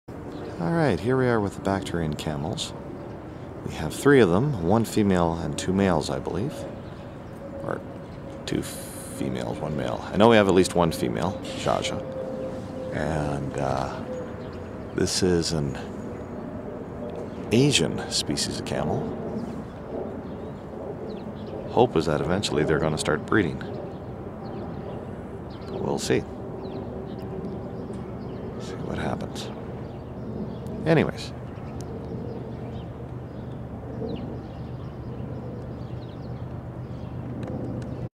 Bactrian Camels
The bactrian camels at the Calgary Zoo, March 3, 2012